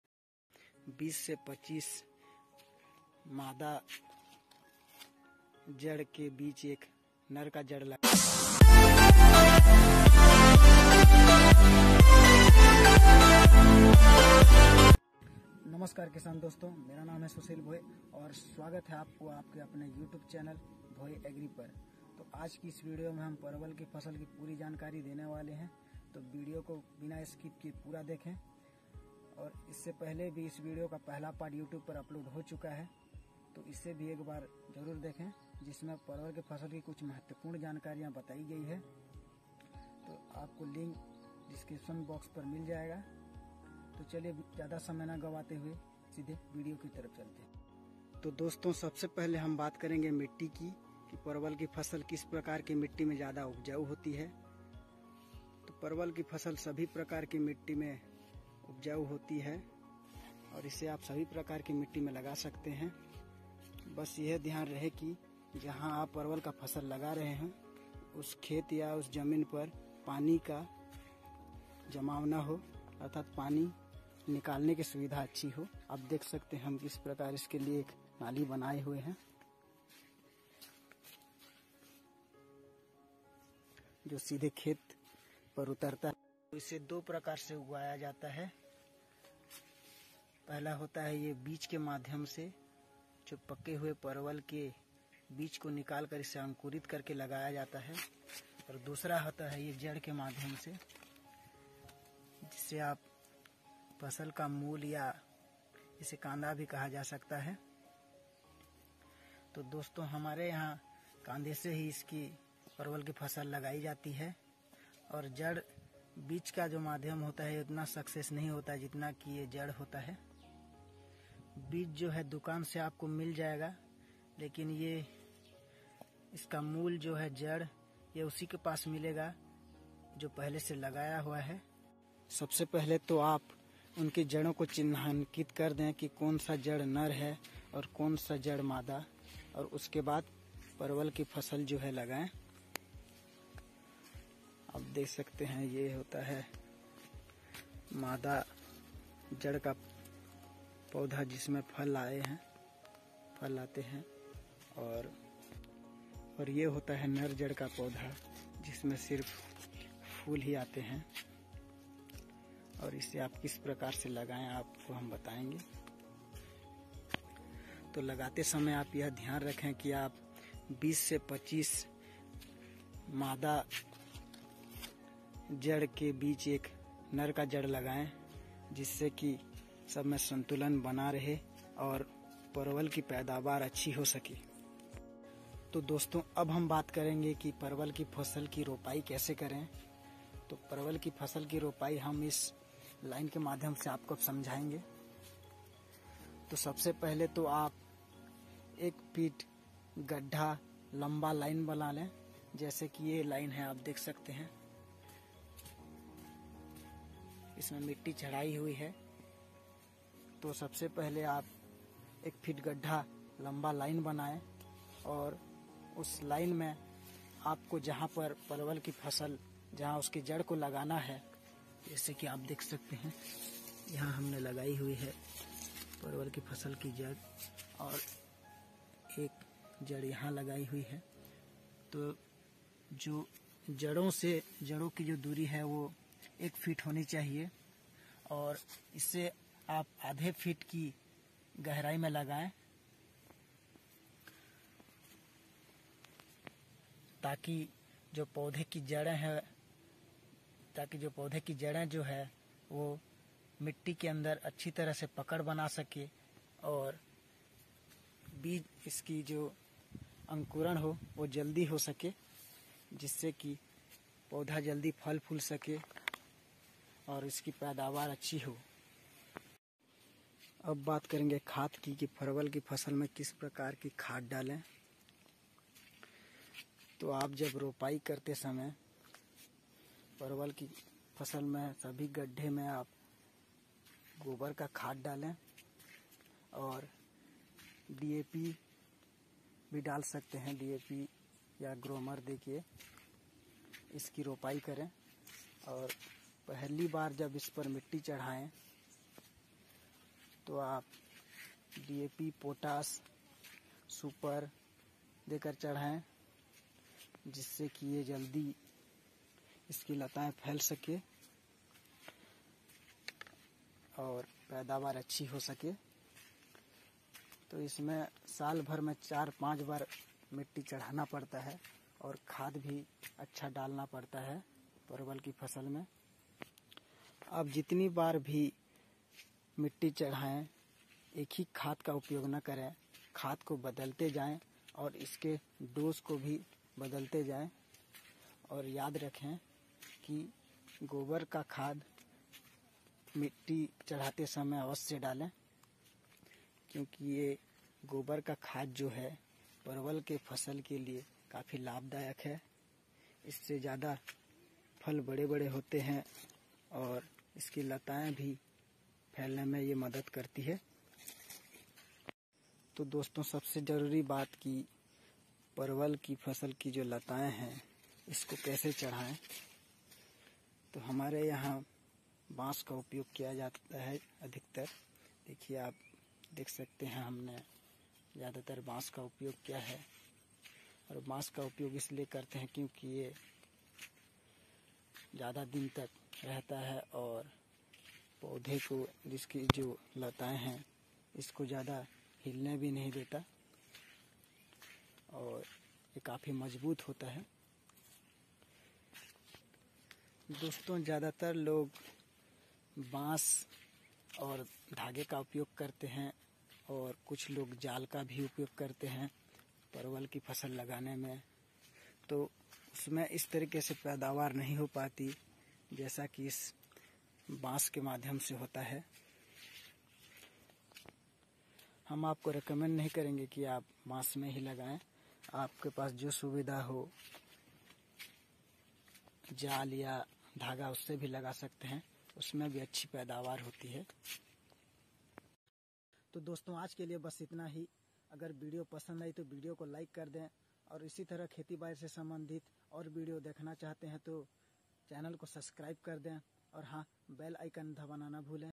परवल की खेती कैसे करें(परवल लगाते समय ये सावधानी रखें)pointed gourd
parwal ki fasal ki Puri jankari
ऐसे लगाएं परवल
परवल फसल की पूरी जानकारी
परवल उगाने का सही तरीका